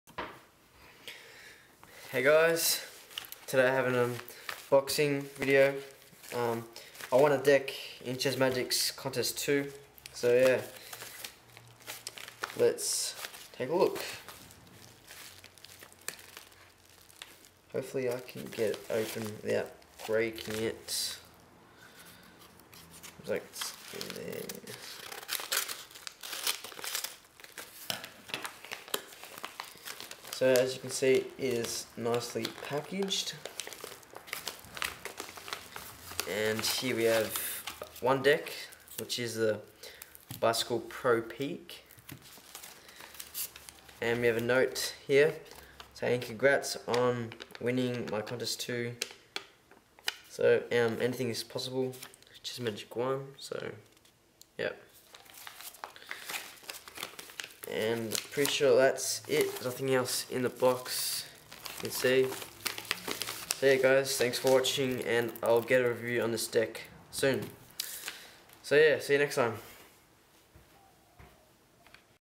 Chezmagic1 Unboxing Video
This is an unboxing of a deck that i won in chezmagic1's contest 2 i used my other account gamerzvx